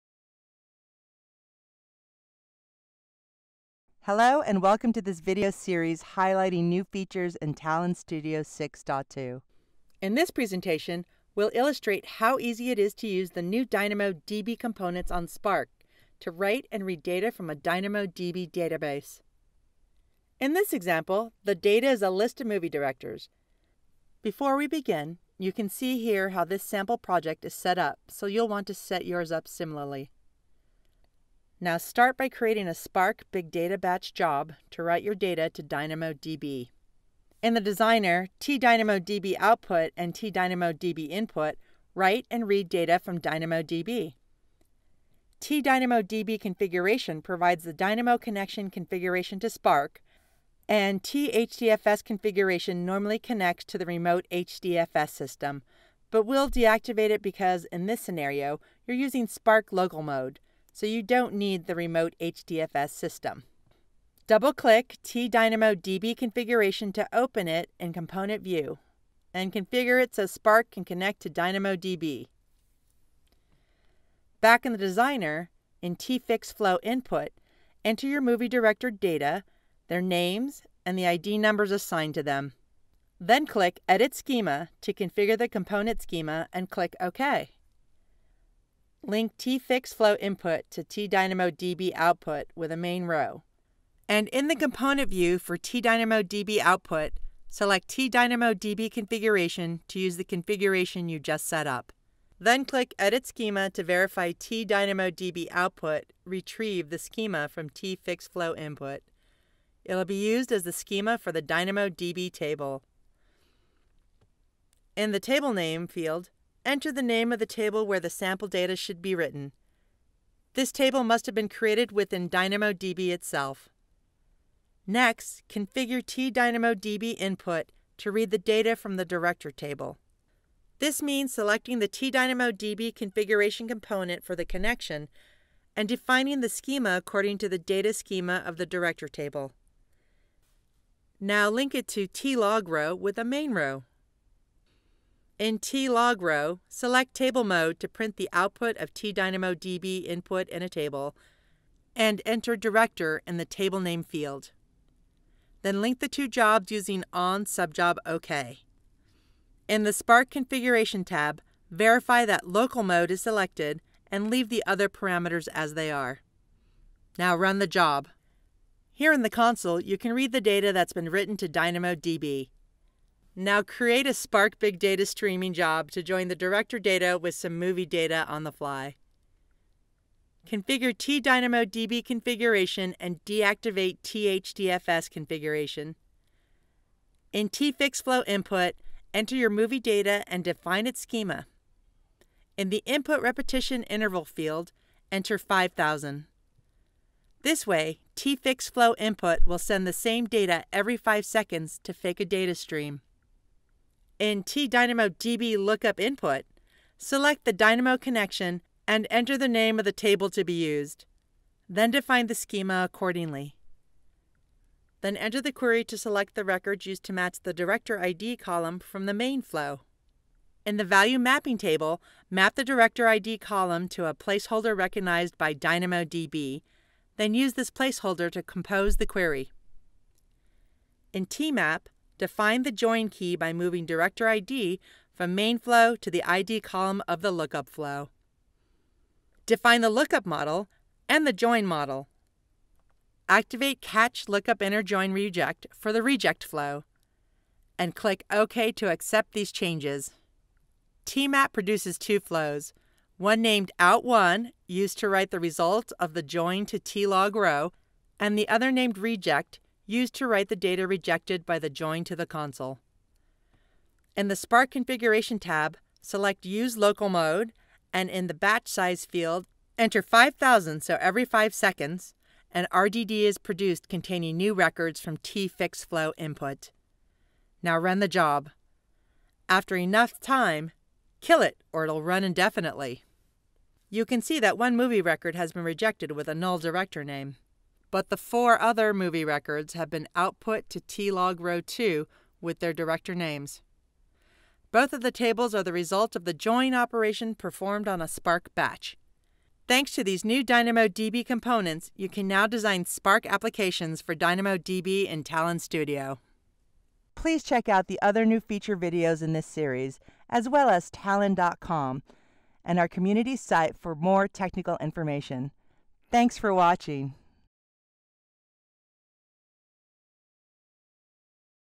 How to design Apache Spark applications to manage DynamoDB databases
This video describes how to graphically design Spark applications to read, write and streaming data with DynamoDB.